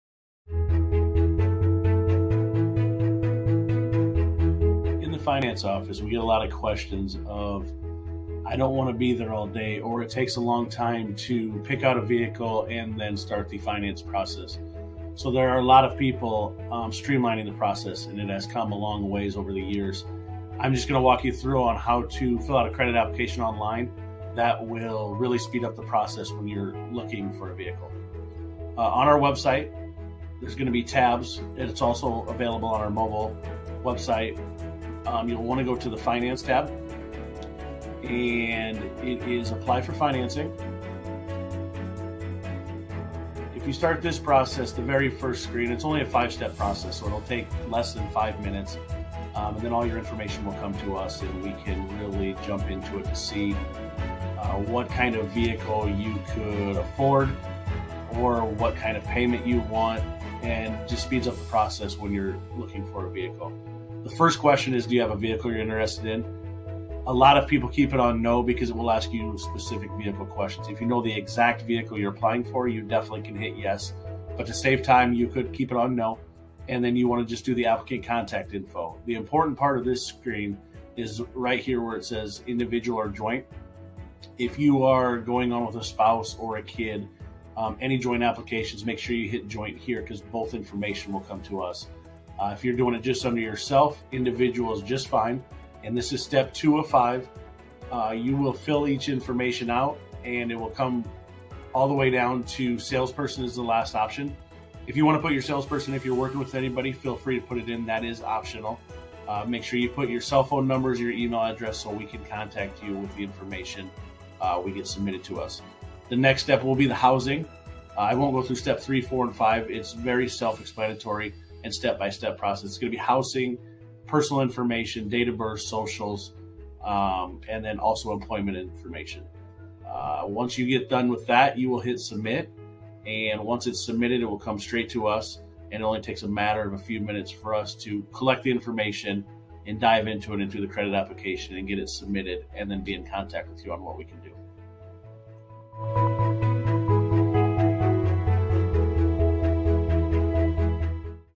Apply for Financing Online
Finance Manager, Jesse Johnson, walks you through how to apply for financing online at kchev.com. Start the process from home, in five minutes or less.